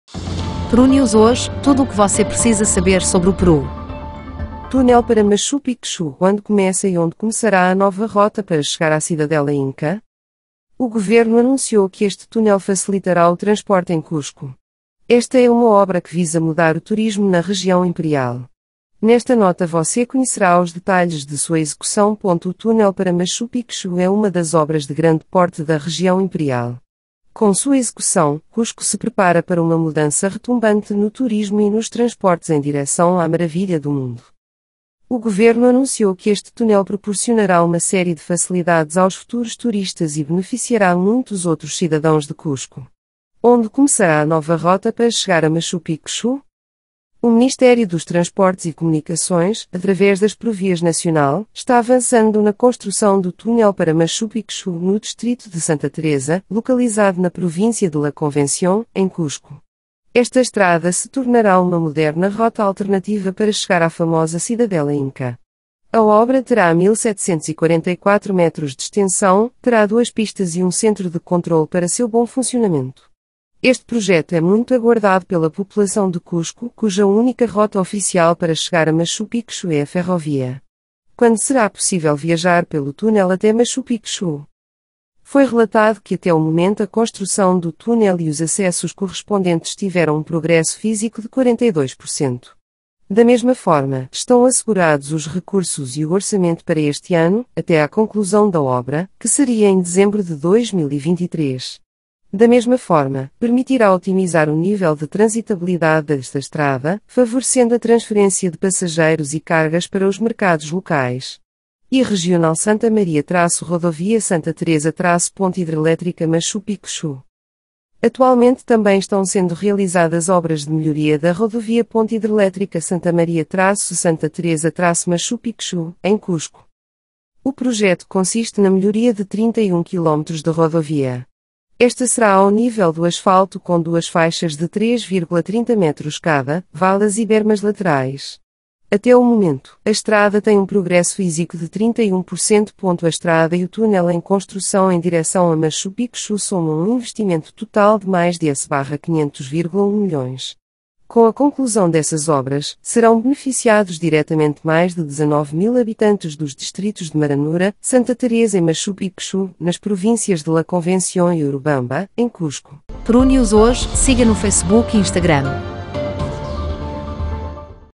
🇧🇷 Túnel para Machu Picchu: quando começa e onde começará a
Túnel para Machu Picchu: quando começa e onde começará a nova rota para chegar à cidadela Inca?
O Governo anunciou que este túnel facilitará o transporte em Cusco. Esta é uma obra que visa mudar o turismo na região imperial. Nesta nota você conhecerá os detalhes de sua execução.O túnel para Machu Picchu é uma das obras de grande porte da região imperial. Com sua execução, Cusco se prepara para uma mudança retumbante no turismo e nos transportes em direção à maravilha do mundo.